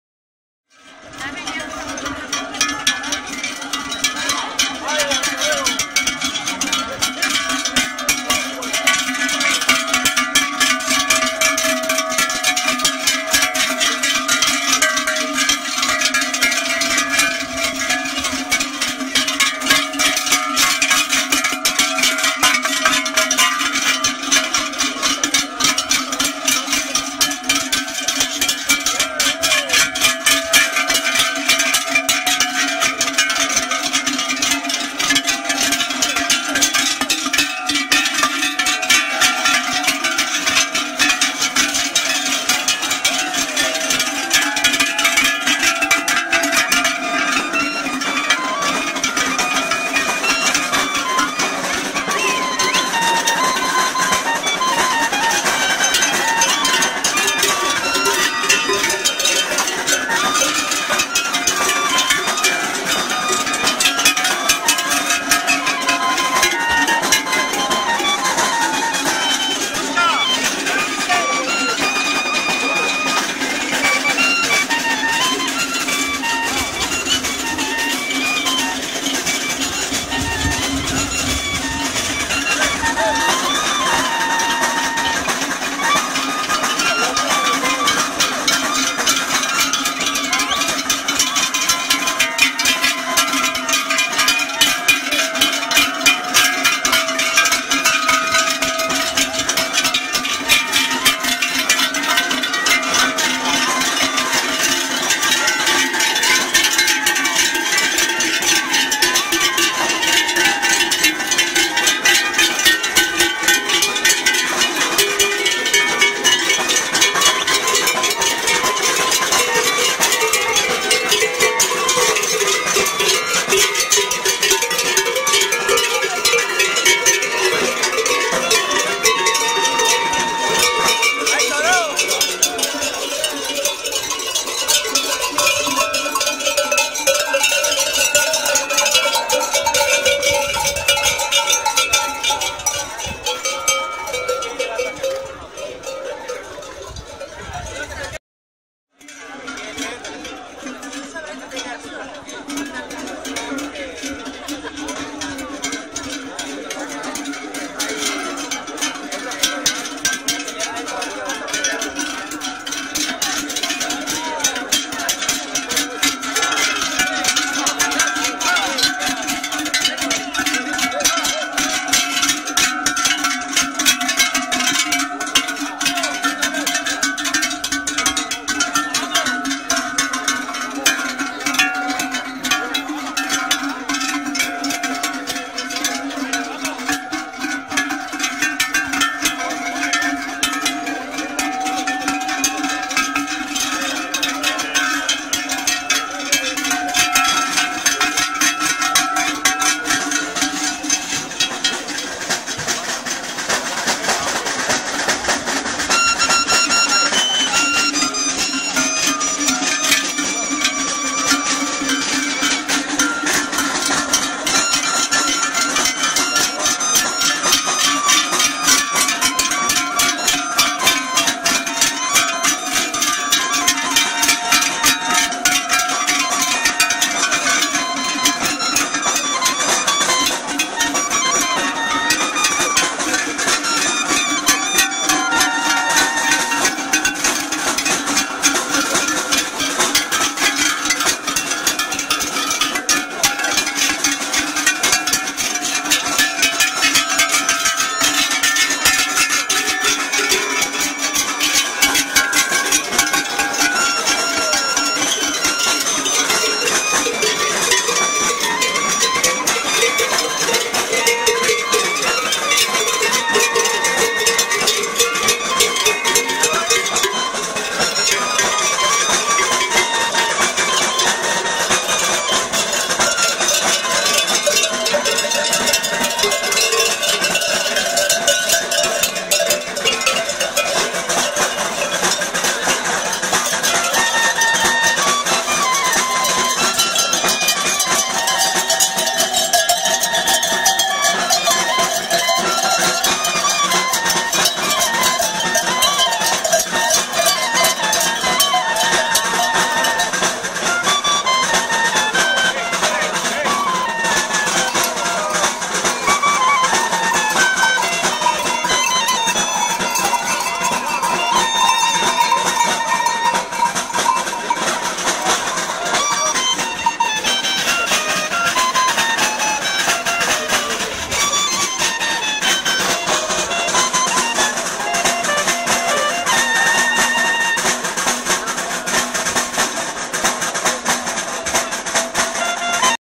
Feria de Santa Lucía 2015, Cabezón de la Sal. (4)
Salida del ganado del recinto ferial Santa Lucía 13 de diciembre 2015.